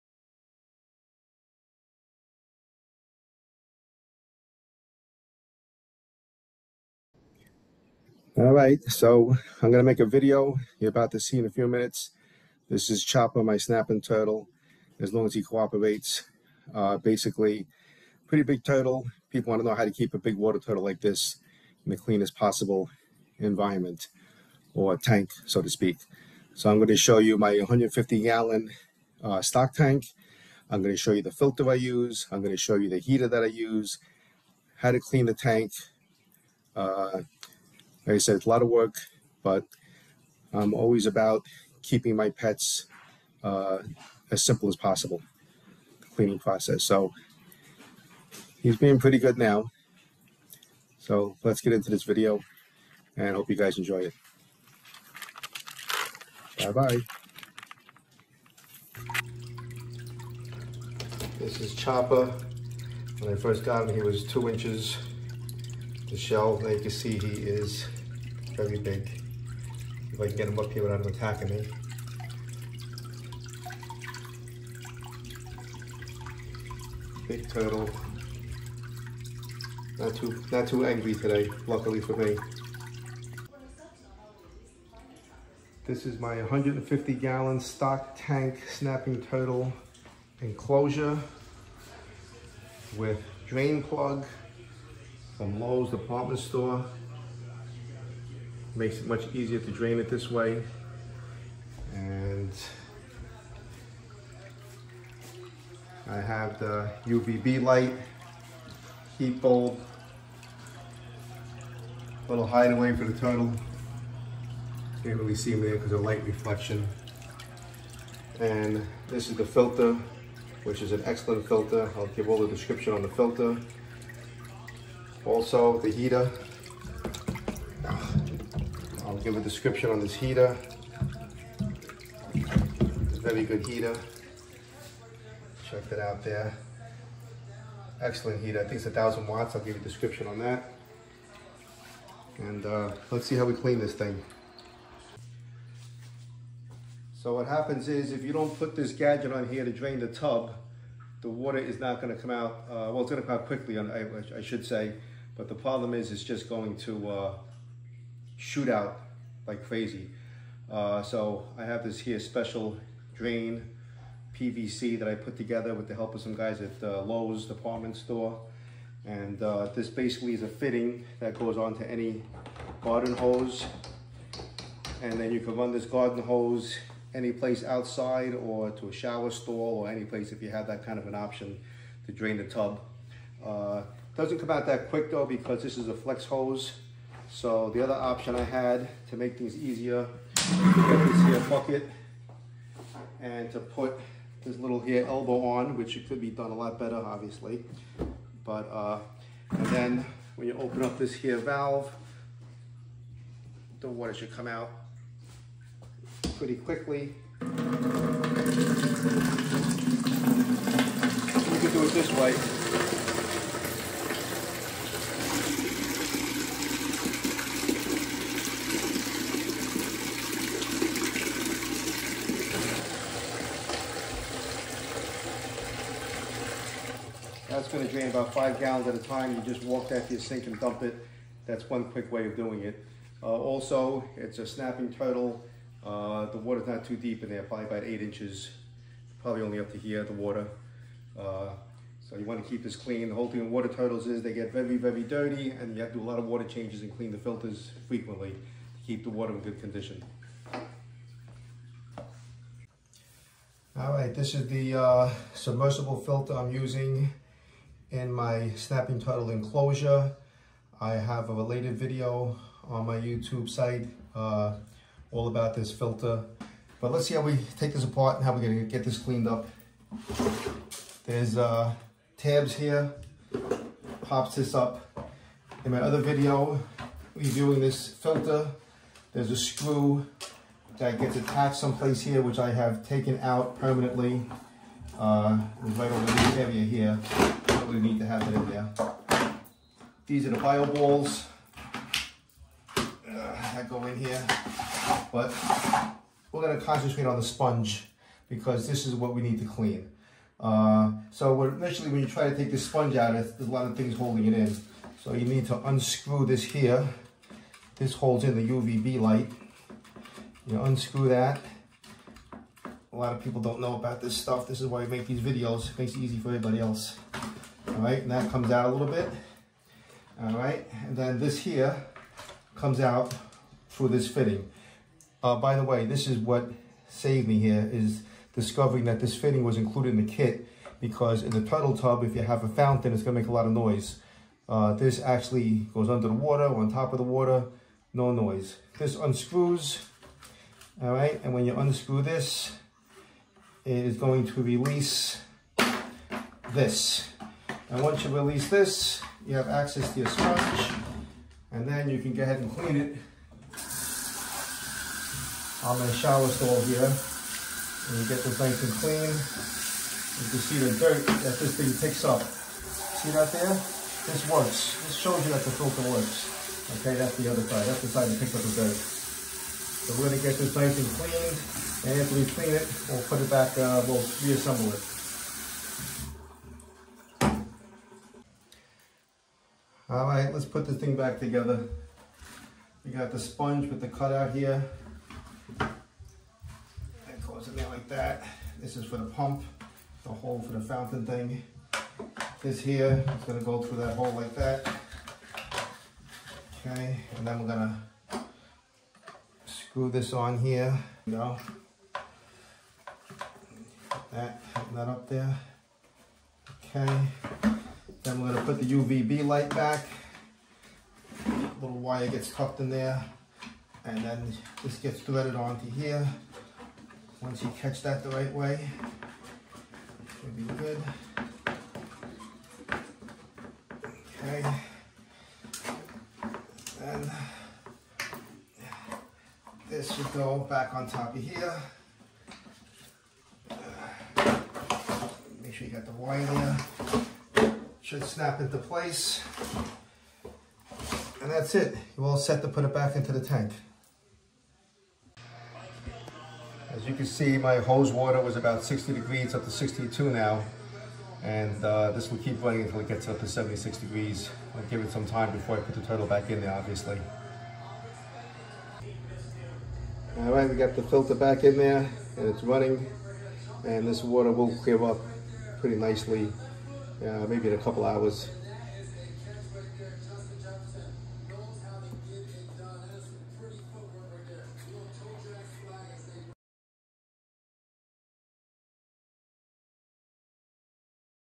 Easy Clean Large Turtle Set Up
In this video I'll show you the one of the easiest ways to keep a large water turtle's tank clean. I have in in a Rubbermaid 150 gallon stock tank with drain plug. The entire drain plug can be dismantled and taken to a local hardware store for the proper PVC elbow / garden hose fitting. The heater I use is an excellent one. Its called YaYaHappy submersible 1000w heater its about 60.00 on Amazon. For filtration I'm using a VIVOHOME 660 GPH submersible which comes with a bio balls and a UVB light it cost less 100.00 on Amazon.
It took my several months to settle on this set up. Everything else I tried was much more work, including canister filters with the many sponges to rise.

UPDATE see my latest video The Ultimate Turtle Filter